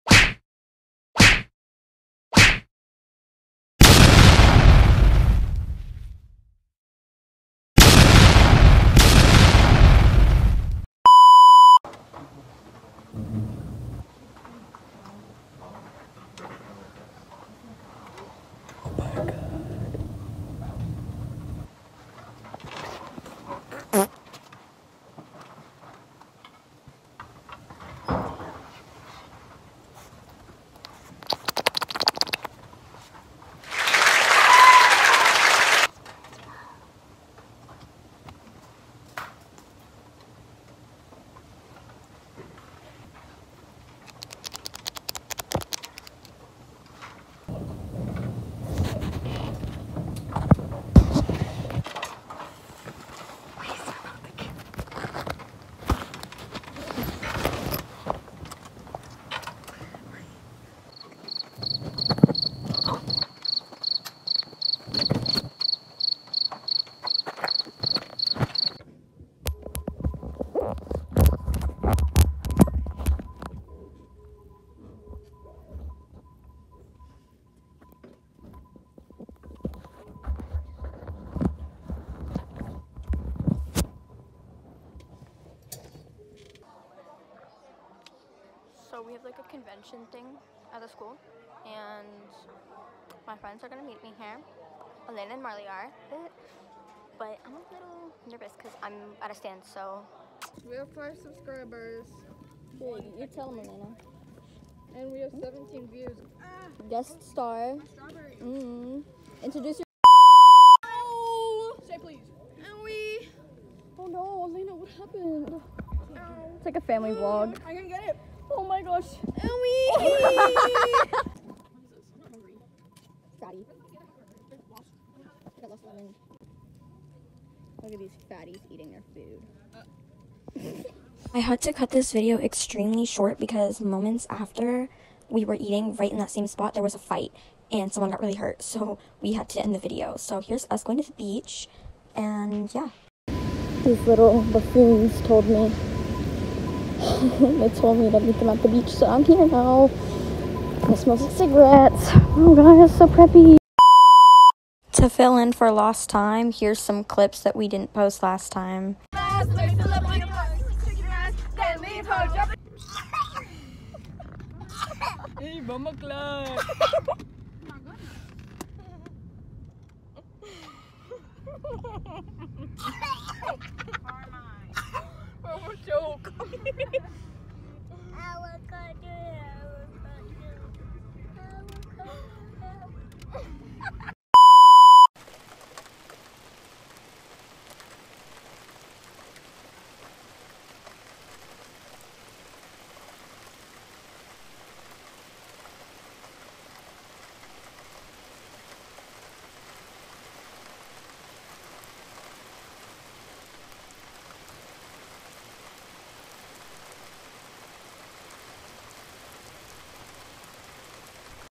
SCHOOL VLOG! (fights and ASMR)
Marz and Alana goof off, Cara is our newest guest star!

Note: we do NOT condone any violence of any kind.

I don’t have much more of a description, but please let’s get this video to 5 likes!